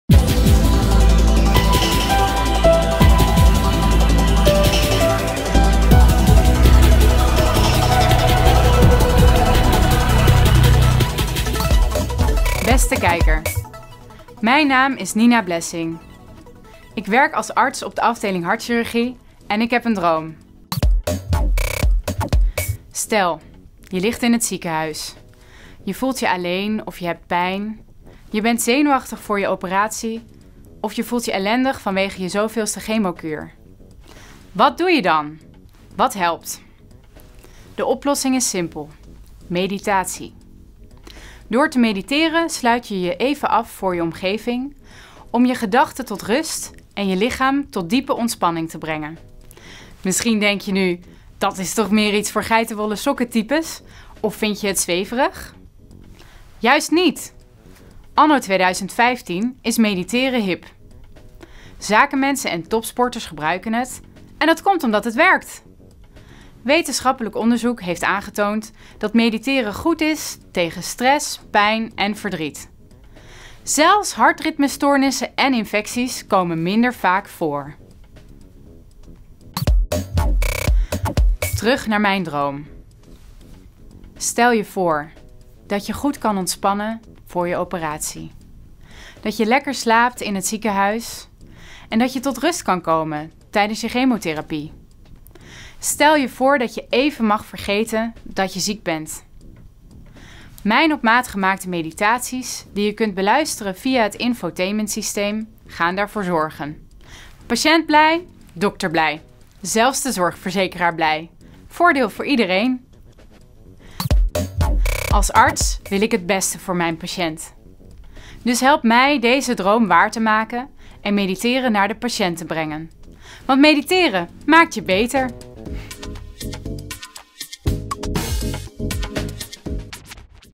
Ureka2015 Nina Meditatie voor patiënten
Patiënten in het ziekenhuis kunnen stress, pijn, verdriet en complicaties verminderen door te mediteren! Via het multi media systeem aan het ziekenhuisbed kunnen patiënten luisteren naar geleide meditaties en zo even vergeten dat ze ziek zijn. Dit draagt wetenschappelijk bewezen bij aan een sneller en beter ziekteherstel.